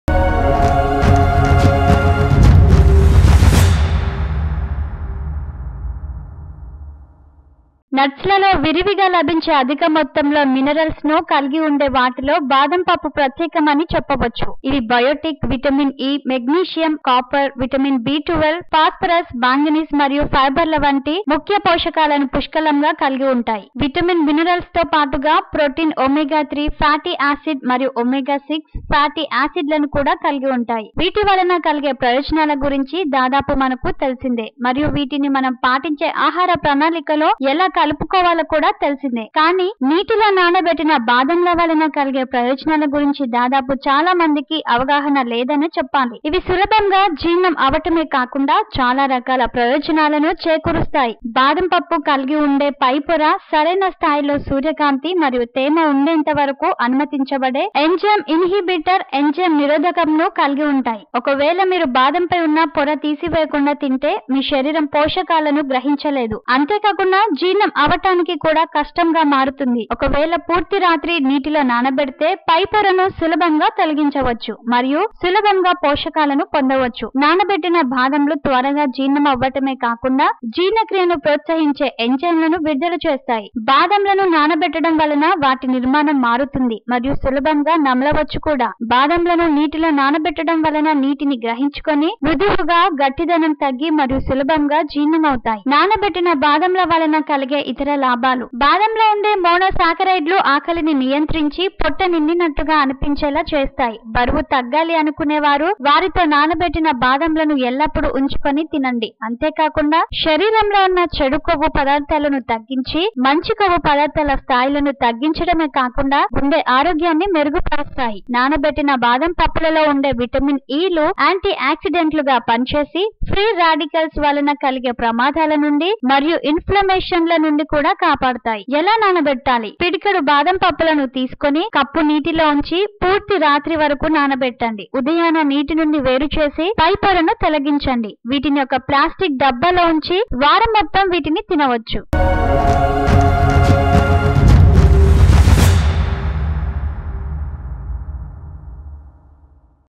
నానబెట్టిన పచ్చి బాదంల వలన కలిగే ప్రయోజనాలు...//X9MEDIA
నట్స్ లలో విరివిగా లభించే, అధిక మొత్తంలో మినరల్ లను కలిగి ఉండే వాటిలో బాదంపప్పు ప్రత్యేకమని చెప్పవచ్చు. ఇవి బయోటిన్, విటమిన్ 'E', మెగ్నీషియం, కాపర్, విటమిన్ 'B12', పాస్పరస్, మాంగనీస్ మరియు ఫైబర్ ల వంటి ముఖ్య పోషకాలను పుష్కలంగా కలిగి ఉంటాయి. విటమిన్, మినరల్ లతో పాటుగా, ప్రోటీన్, ఒమేగా-3 ఫాటీ ఆసిడ్ మరియు ఒమేగా-6 ఫాటీ ఆసిడ్ లను కూడా కలిగి ఉంటాయి. వీటి వలన కలిగే ప్రయోజనాల గురించి దాదాపు మనకు తెలిసిందే మరియు వీటిని మనం పాటించే ఆహార ప్రణాళికలోఎలా కలుపుకోవాలో కూడా తెలిసిందే. కానీ, నీటిలో నానబెట్టిన బాదంల వలన కలిగే ప్రయోజనాల గురించి దాదాపు చాలా మందికి అవగాహన లేదనే చెప్పాలి. ఇవి సులభంగా జీర్ణం అవటమే కాకుండా, చాలా రకాల ప్రయోజనాలను చేకూరుస్తాయి.
బాదం పప్పు కలిగి ఉండే పై పొర, సరైన స్థాయిలో సూర్యకాంతి మరియు తేమ ఉండేంత వరకు అనుమతించబడే ఎంజైమ్ ఇన్హిబిటర్ (ఎంజైమ్ నిరోధకం)ను కలిగి ఉంటాయి. ఒకవేళ మీరు బాదంపై ఉన్న పొర తీసివేయకుండా తింటే, మీ శరీరం పోషకాలను గ్రహించలేదు. అంతేకాకుండా, జీర్ణం అవటానికి కూడా కష్టంగా మారతుంది. ఒకవేళ పూర్తి రాత్రి నీటిలో నానబెడితే, పై పొరను సులభంగా తొలగించవచ్చు మరియు సులభంగా పోషకాలను పొందవచ్చు. నానబెట్టిన బాదంలు త్వరగా జీర్ణమవటమే కాకుండా, జీర్ణక్రియను ప్రోత్సహించే ఎంజైమ్ లను విడుదల చేస్తాయి.

బాదంలను నానబెట్టడం వలన వాటి నిర్మాణం మారుతుంది మరియు సులభంగా నమలవచ్చు కూడా. బాదంలను నీటిలో నానబెట్టడం వలన నీటిని గ్రహించుకొని, మృదువుగా గట్టిదనం తగ్గి మరియు సులభంగా జీర్ణం అవుతాయి.
నానబెట్టిన బాదంల వలన కలిగే ఇతర లాభాలు
బాదంలో ఉండే మోనోసాకరైడ్ లు ఆకలిని నియంత్రించి, పొట్ట నిండినట్టుగా అనిపించేలా చేస్తాయి. బరువు తగ్గాలి అనుకునే వారు వారితో నానబెట్టిన బాదంలను ఎల్లపుడు ఉంచుకొని తినండి. అంతేకాకుండా, శరీరంలో ఉన్న చెడు కొవ్వు పదార్థాలను తగ్గించి, మంచి కొవ్వు పదార్థాల స్థాయిలను తగ్గించటమే కాకుండా, గుండె ఆరోగ్యాన్ని మెరుగుపరుస్తాయి. నానబెట్టిన బాదంపప్పులలో ఉండే విటమిన్ 'E' లు యాంటీ ఆక్సిడెంట్ లుగా పని చేసి, ఫ్రీ రాడికల్ ల వలన కలిగే ప్రమాదాల నుండి మరియు ఇన్ఫ్లమేషన్ ల నుండి కూడా కాపాడతాయి.
ఎలా నానబెట్టాలి?
పిడికెడు బాదంపప్పులను తీసుకొని, కప్పు నీటిలో ఉంచి పూర్తి రాత్రి వరకు నానబెట్టండి. ఉదయాన నీటి నుండి వేరు చేసి, పై పొరను తొలగించండి. వీటిని ఒక ప్లాస్టిక్ డబ్బాలో ఉంచి వారం మొత్తం వీటిని తినవచ్చు.